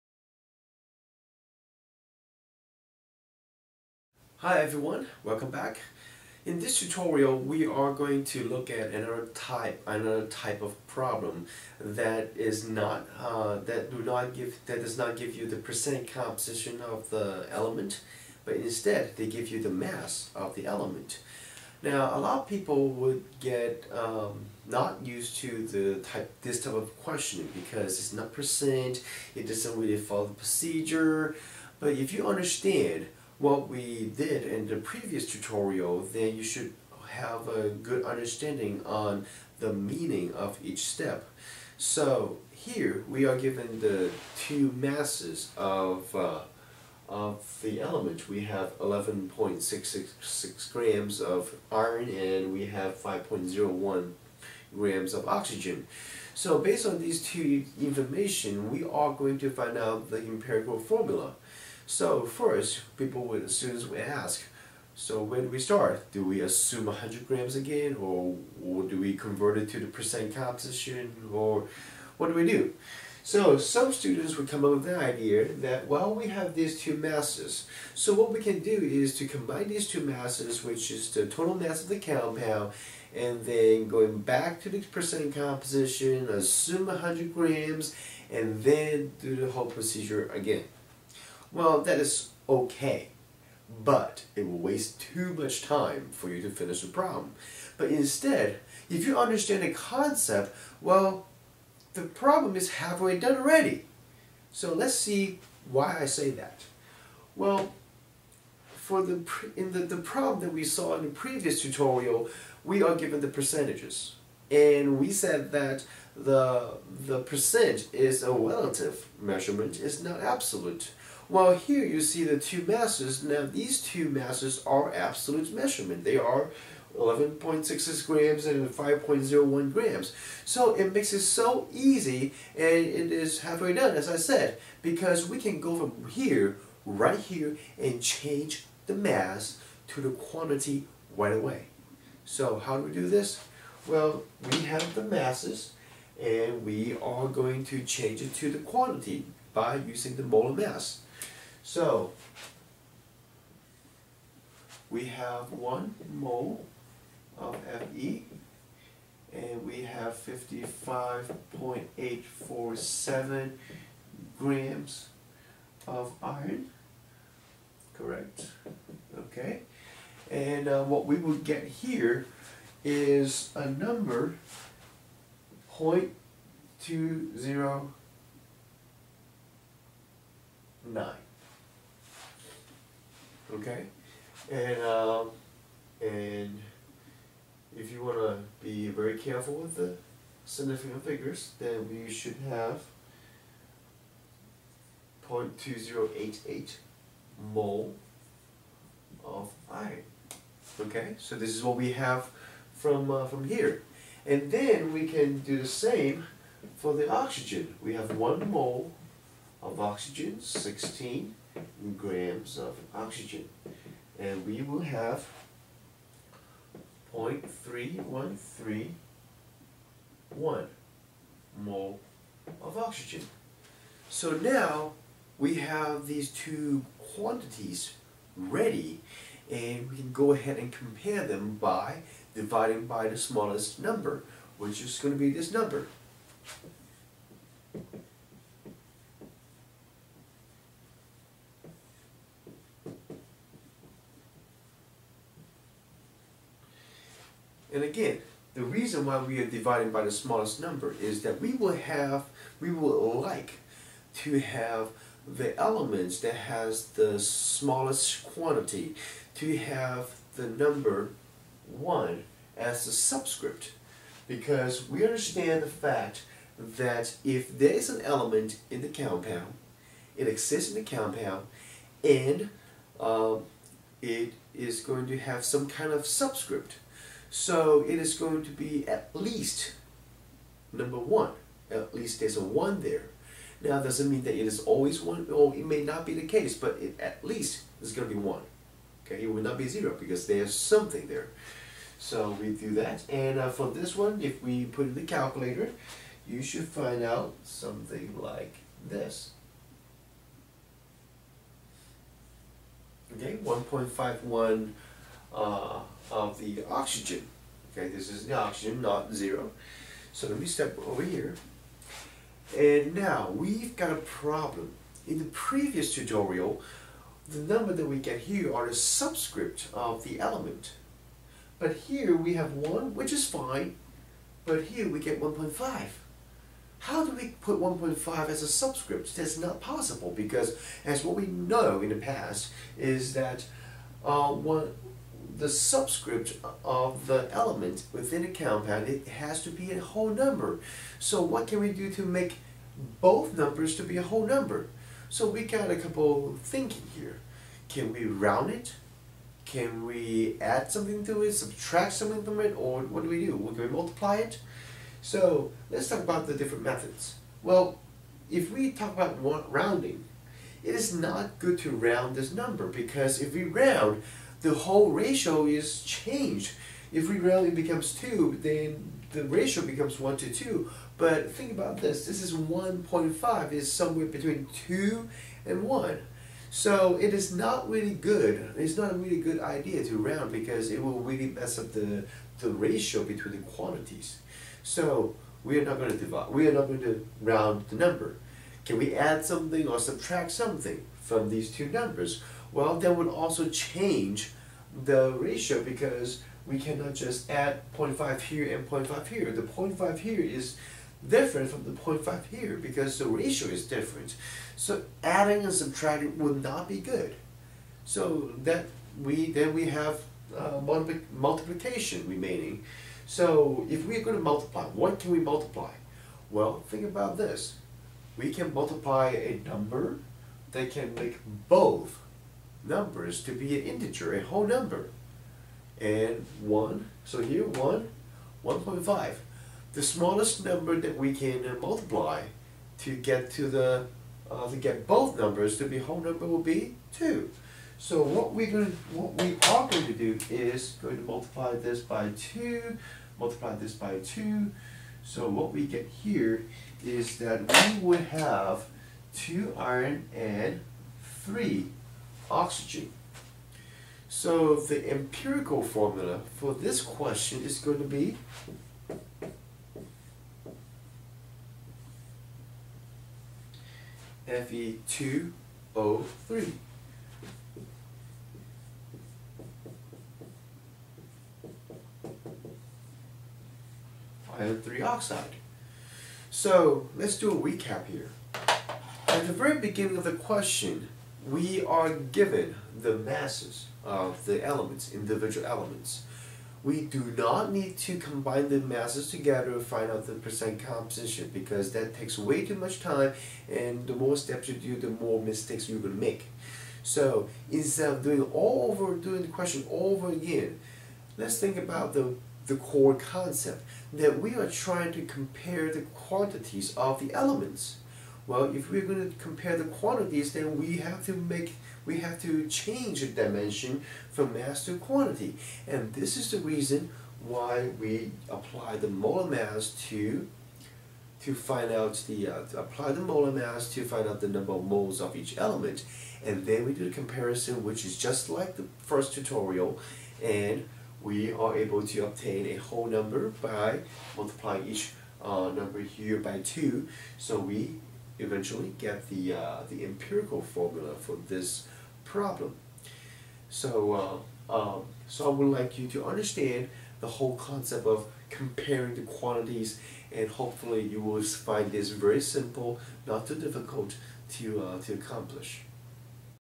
[HD] Empirical Formula - Part 2
In this tutorial, you will learn to find the empirical formula by using the given masses of the elements.